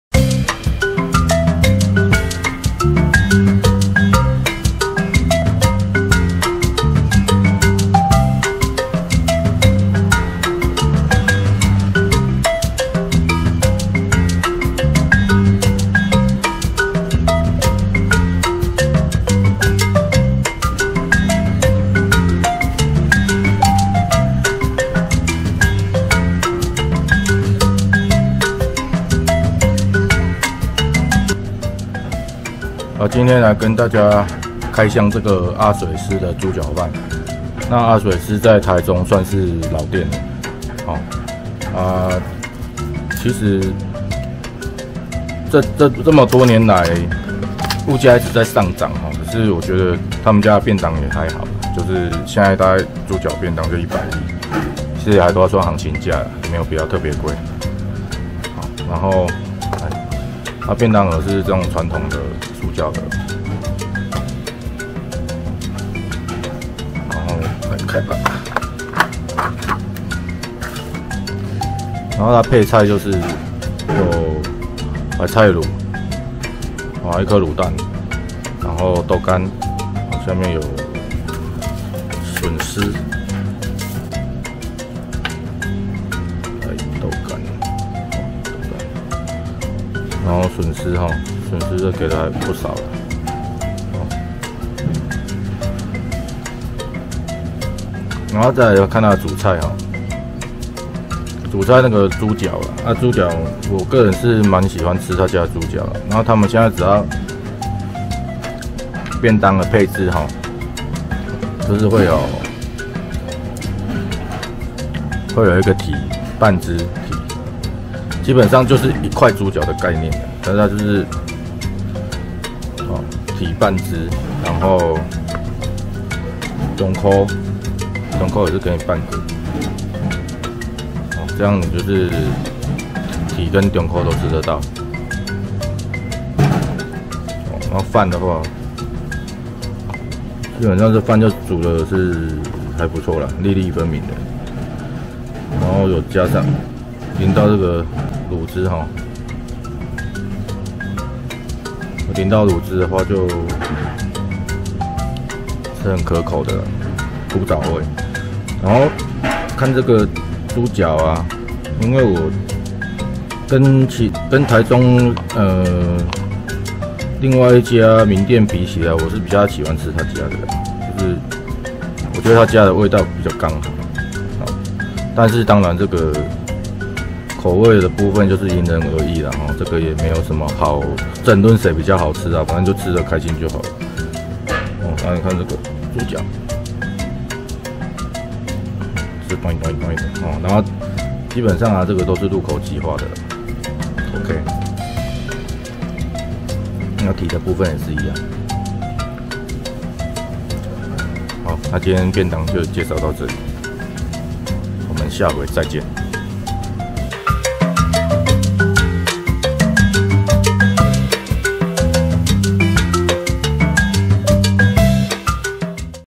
【電冰箱美食分享頻道】 大家來吃便當13A-阿水獅豬腳便當
這家店其實冰箱真的也寫了N遍了，不過吃來吃去還是這家口味最好。
防疫期間只提供外帶服務，不想出門可以找吳柏毅~~~
阿水獅豬腳大王 公園店(總店)
400台中市中區公園路1號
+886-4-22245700
營業時間:10:00~21:00
公休日:無